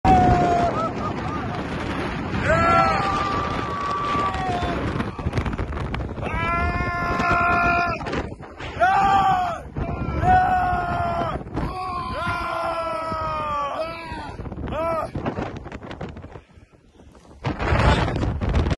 Climbing in shorts to Mount Beshtau( Method Wim Hof)
We with the help of the only certified instructor Alex Maltsev of the method Wim How in Russia climbing in shorts to Mount Beshtau. It's was amazing, friends.